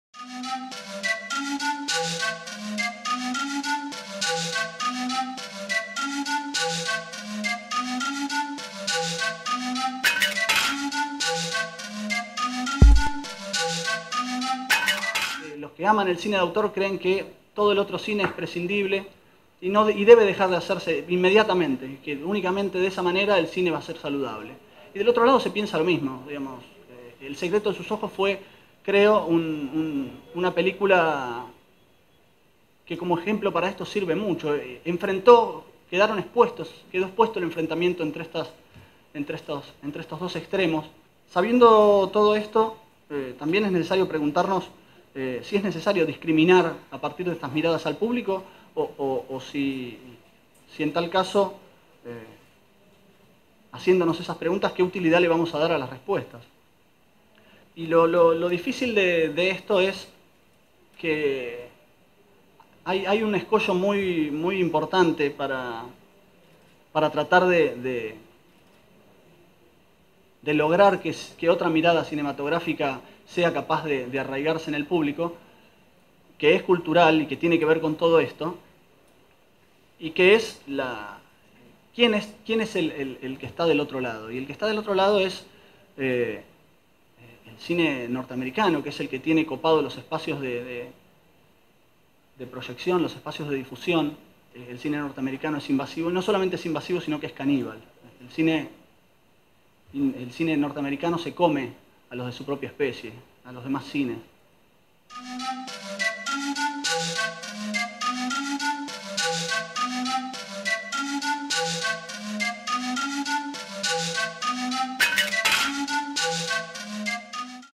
MICA - Juan Pablo Cinelli, crítico de cine
El crítico de cine se presentó en el panel "El espectador cinematográfico" que se realizó el sábado 4 de junio de 2011 durante el primer Mercado de Industrias Culturales Argentinas (MICA)